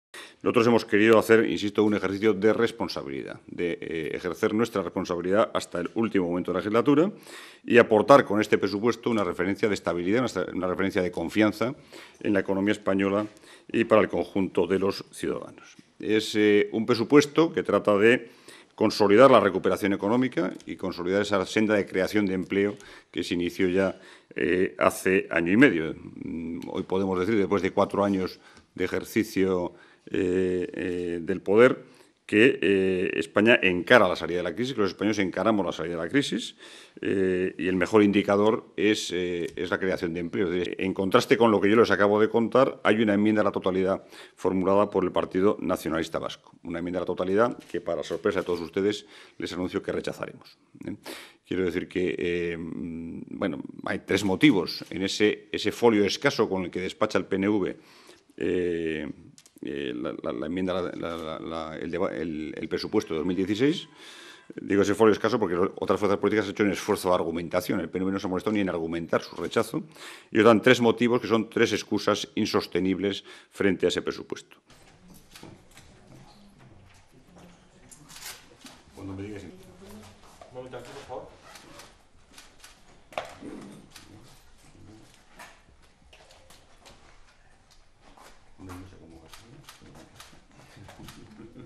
Barreda alaba proyecto Presupuestos Estado 2016
El portavoz adjunto del grupo popular en el Congreso de los diputados, Leopoldo Barreda, ha calificado de ejercicio de responsabilidad el proyecto de Presupuestos del Estado para 2016 presentado por el Gobierno del PP y ha denunciado las "excusas insostenibles" del PNV para presentar una enmienda a la totalidad. Barreda le ha pedido a los nacionalistas que expliquen su rechazo a una inversión "récord" del Estado en Euskadi.